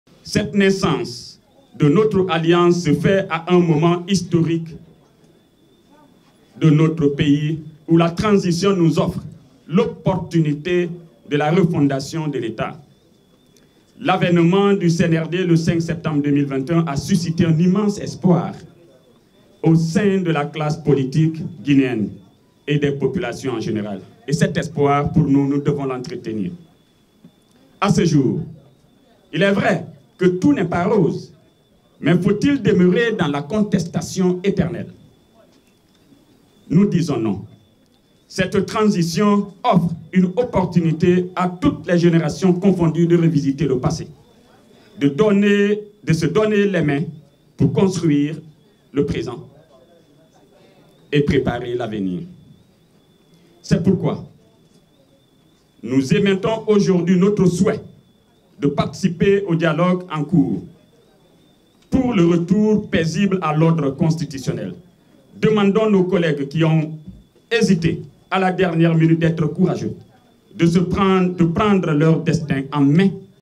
POLITIQUE : Alliance Pour la République APR, prête pour le cadre de dialogue inter Guinéen
Guinée : Chronogramme de la transition, APR veut participer au cadre de dialogue